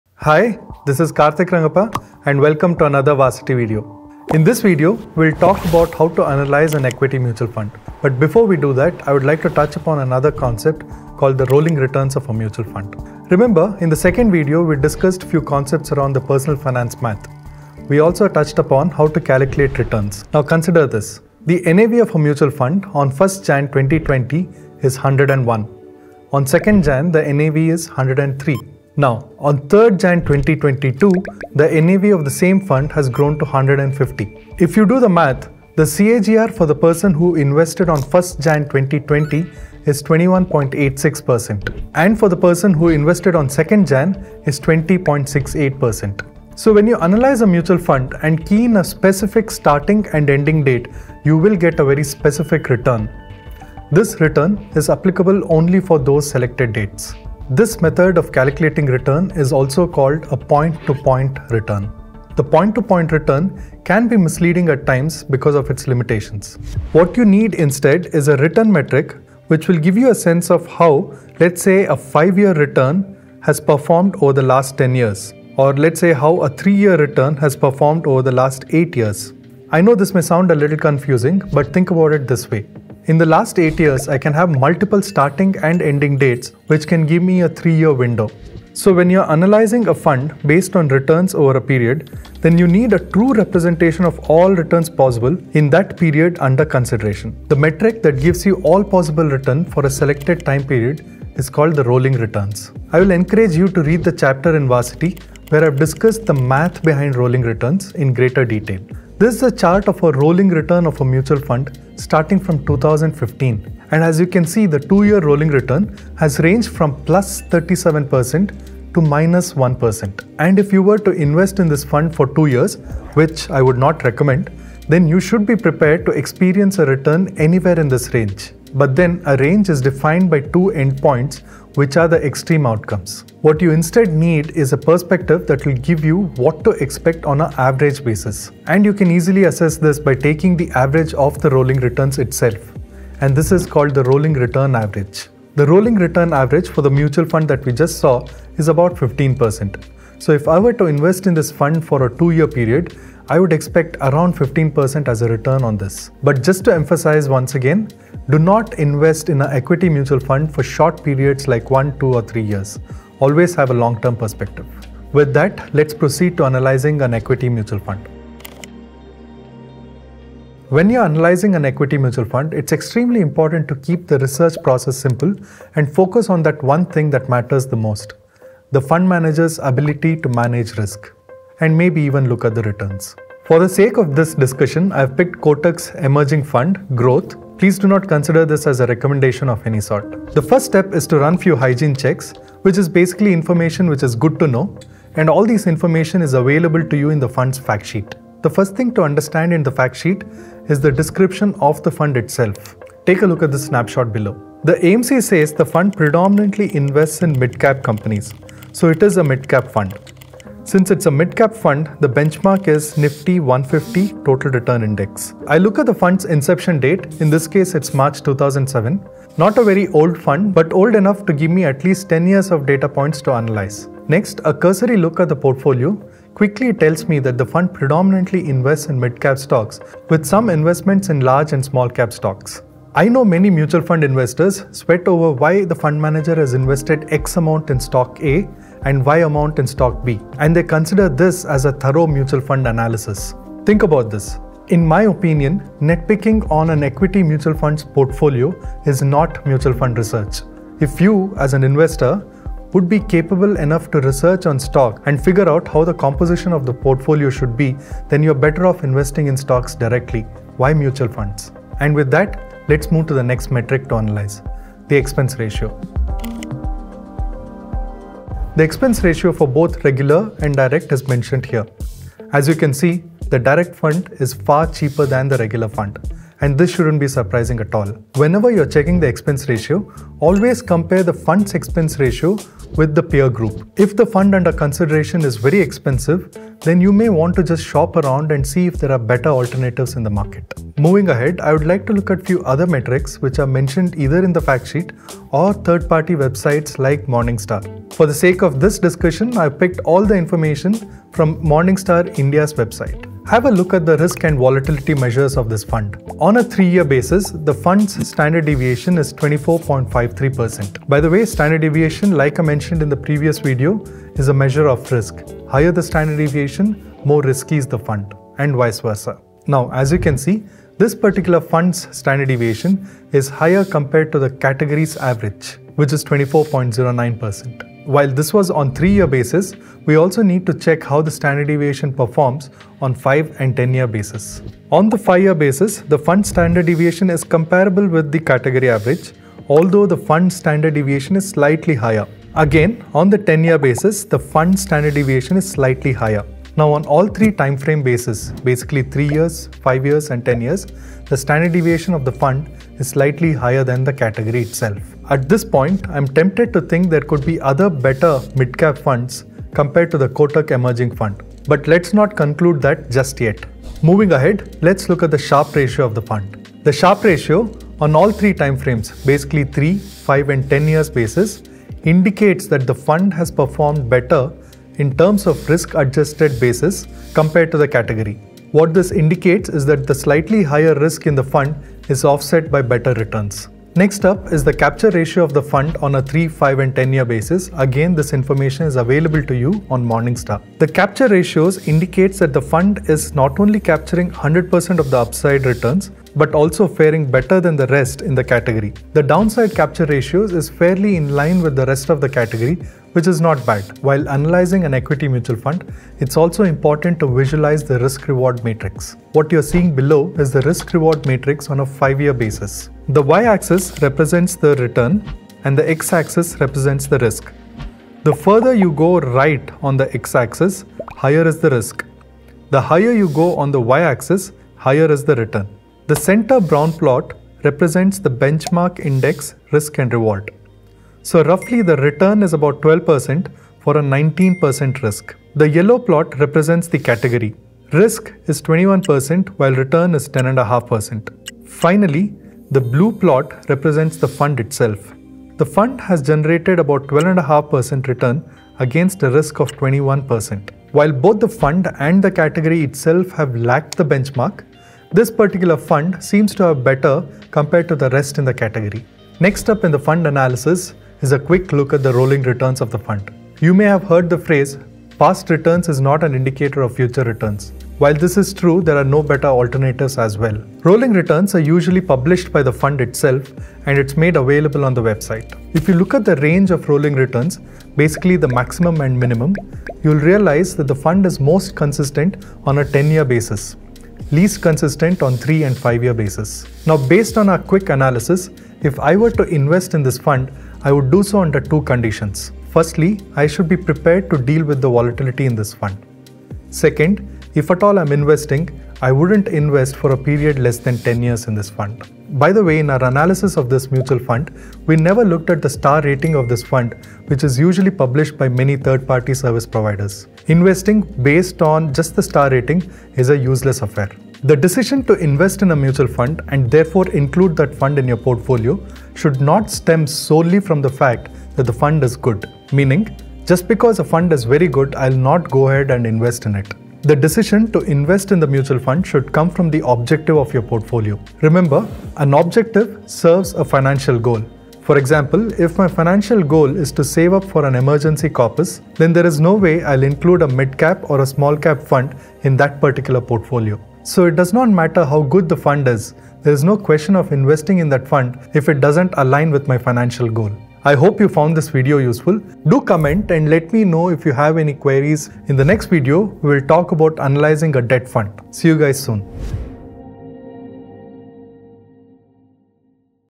How to analyze an equity mutual fund?
Let's say you have shortlisted a couple of equity mutual funds, how do you choose between those? While it's almost impossible to predict which fund will do better, you can ensure that you can pick funds that are well managed. Also, picking funds just by looking at the past return is a recipe for disaster.

In this video, Karthik explains:
1. Why you shouldn't just look at point to point returns and instead look at rolling returns
2. How do understand the risk and return trade-off of funds